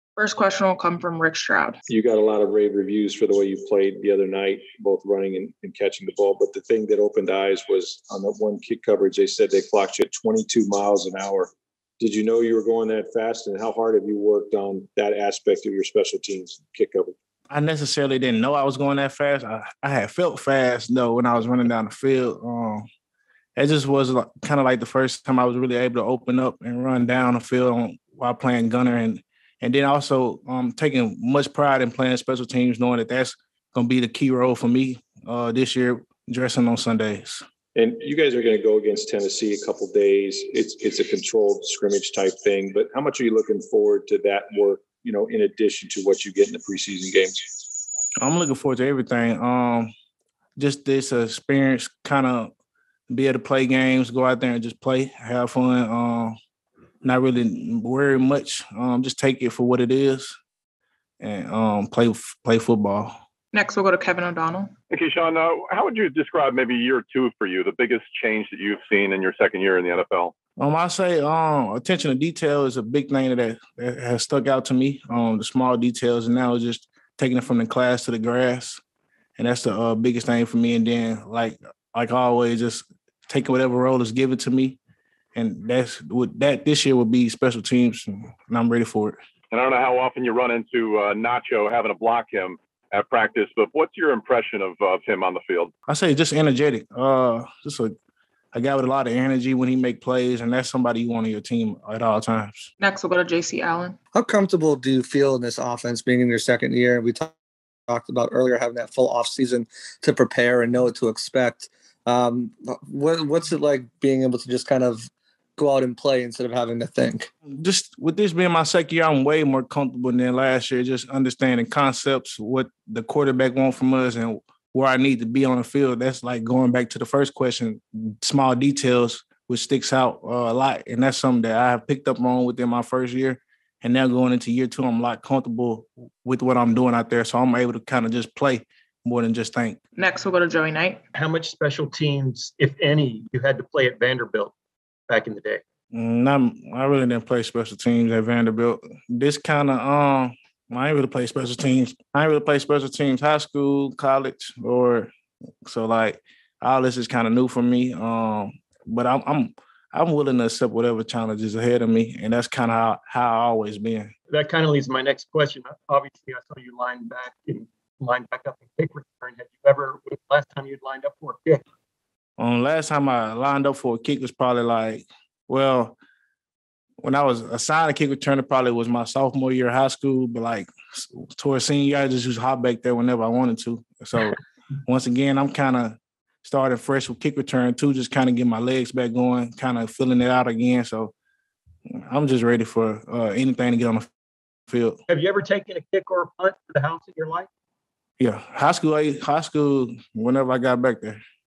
Buccaneers RB Ke'Shawn Vaughn on Bucs Offense, 2021 Season
Bucs RB Ke'Shawn Vaughn press conference!

About Tampa Bay Buccaneers Press Conference: Tampa Bay Buccaneers running back Ke'Shawn Vaughn discusses the 2021 NFL season playing special teams, preseason games, and his incredible speed! Footage courtesy of and provided by the Tampa Bay Buccaneers organization.